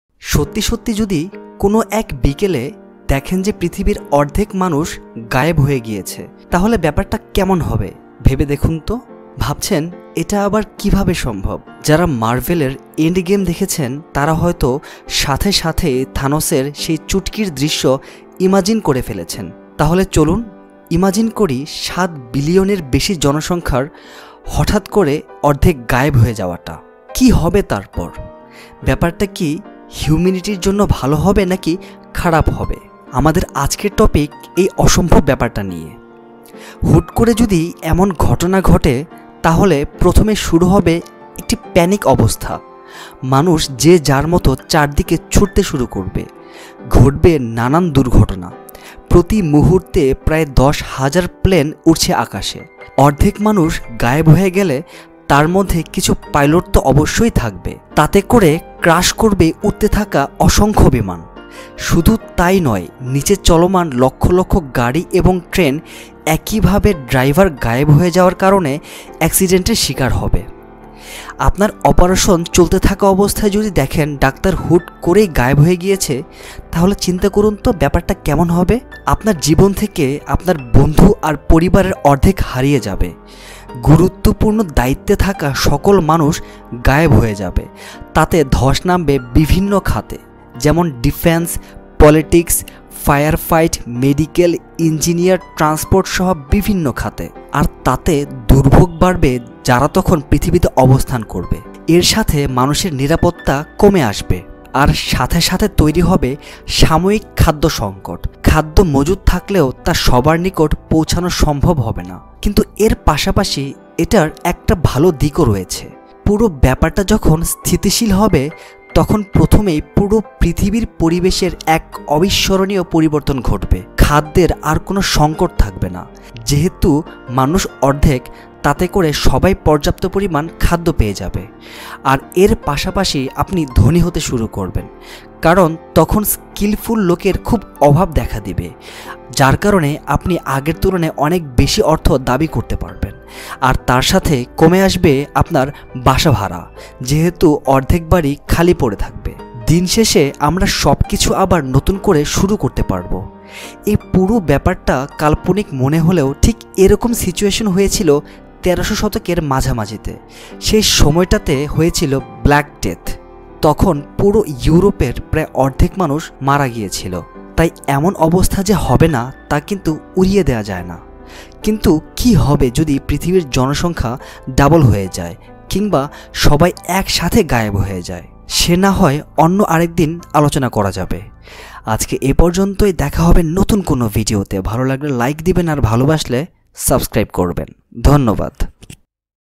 যদি পৃথিবীর অর্ধেক মানুষ গায়েব হয়ে যায়! শুধুই কাল্পনিক? What If Half the Population of Earth vanish?
সত্যি সত্যি যদি কোনো এক বিকালে দেখেন যে পৃথিবীর অর্থেক মানুষ গায়েব হয়ে গেছে, তাহলে বেপারটা কেমন হবে ভেবে দেখুন তো। ভাবছেন এটা আবার কিভাবে সম্ভব। যারা মার্ভেল এর এন্ড গেইম দেখেছেন তারা হয়তো সাথে সাথেই থানোস এর সেই চুটকির দৃশ্য ইমাজিন করে ফেলেছেন।
চলুন ইমাজিন করি ৭ বিলিয়নে এর বেশি জনসংখ্যার হঠাৎ গায়েব হওয়ে যাওয়ার দৃশ্য। কি হবে তার পর? বেপারটা কি হিউমেনিটির জন্য ভালো হবে নাকি খারাপ। আমাদের আজকের টপিক থানোস এর সেই চুটকিকে ঘিড়ে।

হুট করে এমন ঘটনা ঘটলে প্রথমে শুরু হবে এক প্যনিক অবস্থা। মানুষ যে যার মত চারদিকে ছুটতে শুরু করবে। ঘটবে নানান দুর্ঘটনা। প্রতি মুহূর্তে প্রায় ১০ হাজার প্লেন উড়ছে আকাশে। অর্ধেক মানুষ গায়েব হয়ে গেলে তার মধ্যে কিছু পাইলট তো অবশ্যই থাকবে। তাতে করে ক্রাশ হবে উড়তে থাকা বিমান। শুধু তাই নয় নিচে চলমান লক্ষ লক্ষ গাড়ি এবং ট্রেন একই ভাবে ড্রাইভার গায়েব হয়ে যাওয়া তে এক্সিডেন্ট এর শিকার হবে।
আপনার অপারেশন চলতে থাকা অবস্থাতে যদি দেখেন ডাক্তার হুট করেই গায়েব তাহলে কি হবে চিন্তা করুন তো।
আপনার অর্ধেক বন্ধুরা আর অর্ধেক পরিবার হারিয়ে যাবে আপনার জীবন থেকে।
গুরুত্ব পূর্ন দায়িত্বে থাকা সকল মানুষ গায়েব হয়ে যাবে। তাতে ধষ নামবে বিভিন্ন খাতে। যেমন: ডিফেন্স, পলিটিক, ফায়ার ফাইট, মেডিকাল, ইন্জিনিয়ার, ট্রান্সপোর্ট ইত্যাদি।
আর তাতে দুর্ভোগ বারবে যারা তখন পৃথিবীতে অবস্থান করবে। আর তাতে করে মানুষের নিরাপত্তা কমে যাবে সাথে তৈরি হবে সাময়িক খাদ্য সংকট। খাদ্য মজুদ থাকলেও তা সবার নিকট পৌছানো সম্ভব হবে না।
কিন্ত এর পাশাপাশি এর এক ভালো দিকও রয়েছে। পুরো ব্যাপারটা যখন স্থিতিশীল হবে তখন প্রথমেই পুরো পৃথিবীর পরিবেশের এক অবিষ্মরনীয় পরিবর্তন হবে। খাদ্যের আর কোনো সংকট থাকবে না। যেহেতু মানুষ অর্ধেক তাতে করে সবাই পর্যাপ্ত খাদ্য পেয়ে যাবে।
আর এর পাশাপাশি আপনি ধনী হতে শুরু করবেন। কারন তখন স্কিলফুল লোকের অভাব দেখা দিবে, যার কারনে আপনি আগের তুলনায় অনেক বেশি অর্থ দাবি করতে পারবেন। আর তার সাথে কমে আসবে আপনার বাসা ভাড়া। যেহেতু অর্ধেক বাড়ি খালি পরে থাকবে।
দিন শেষে আমরা সব কিছু নতুন করে শুরু করতে পারবো।
এ পুরো বেপারটা কাল্পনিক মনে হলেও ঠিক এরকম সিচুয়েশনে পরতে হয়েছিল ১৩০০ শতকের মাঝামাঝিতে। সে সময়টাকে বলা হয় ব্লাক ডেথ। তখন পুরো ইউরোপের প্রায় অর্থেক মানুষ মারা  গিয়েছিলো। তাই এমন অবস্থা যে হবে না তা কিন্ত উড়িয়ে দেয়া যায় না।
কিন্ত কি হবে যদি পৃথিবীর জনসংখ্যা ডাবল হয়ে যায় কিংবা সবাই গায়েব হয়ে যায়? সে না হয় অন্য আরেকদিন আলোচনা করবো।
আজকে এ পর্যন্তই, দেখা হবে নতুন কোনো ভিডিওতে।
ভালো লাগলে লাইক দিবেন, আর ভালো বাসলে সাব্সক্রাইব করবেন।